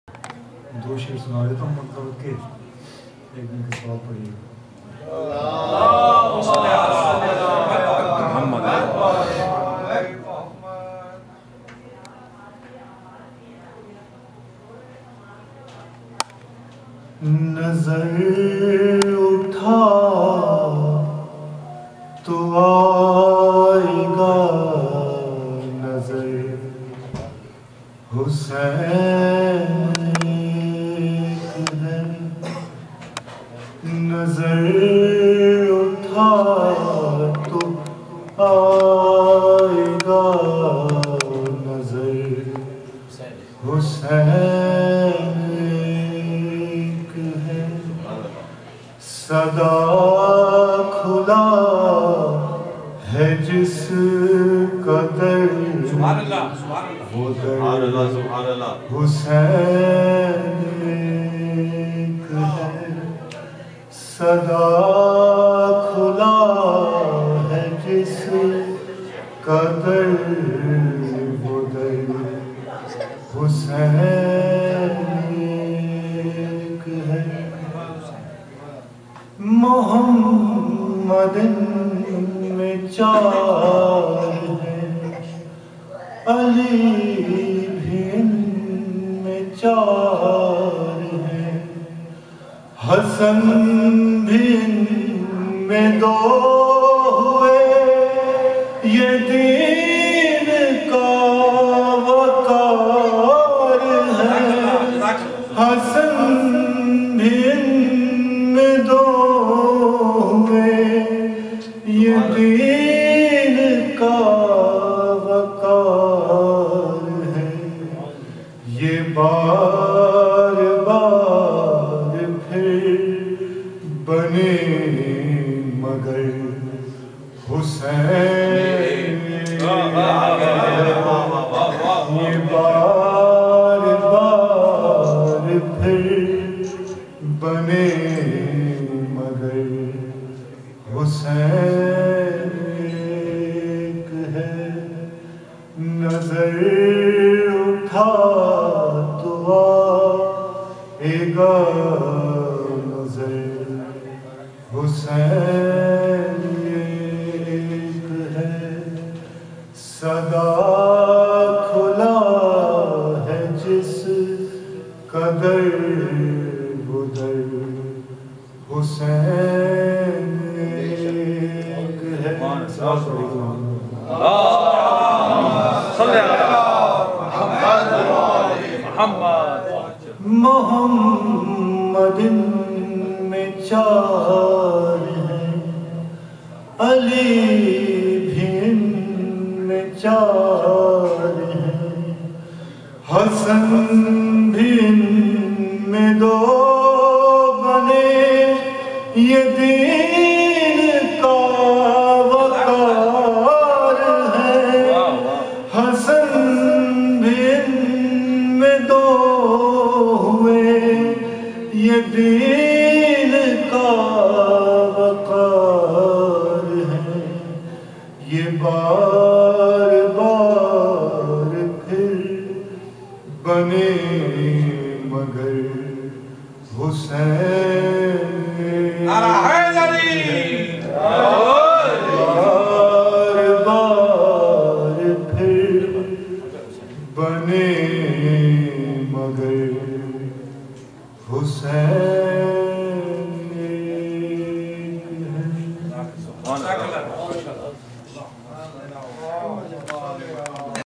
Jashane Wiladat Imam Muhammed Baqir Aleyhis Salam - 1st Rajab at Bayt ul Qaim
Thursday May 1st, 2014 - Jashane Wiladat Imam Muhammed Baqir Aleyhis Salam - 1st Rajab at Bayt ul Qaim